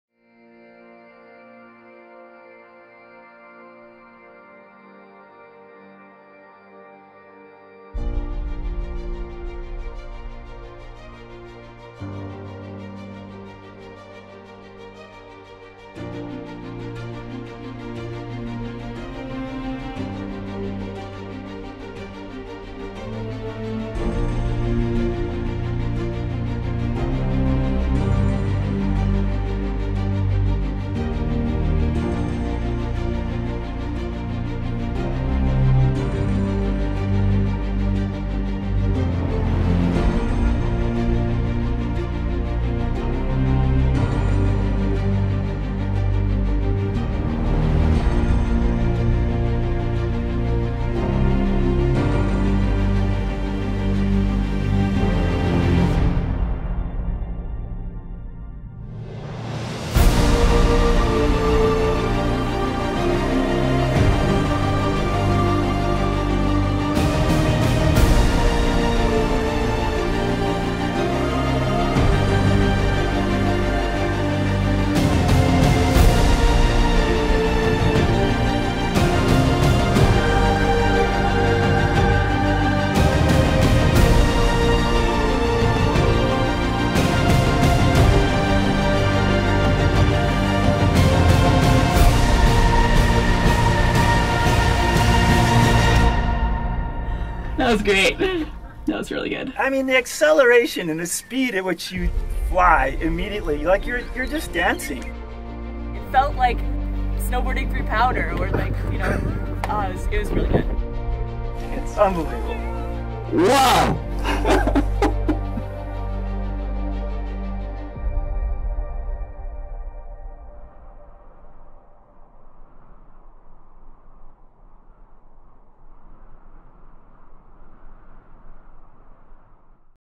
Pivotal | BlackFly Flight
BlackFly is Pivotal’s eVTOL aircraft, offered to a select group of early access customers. Pivotal’s fourth generation aircraft, Helix, is now available to all eligible customers.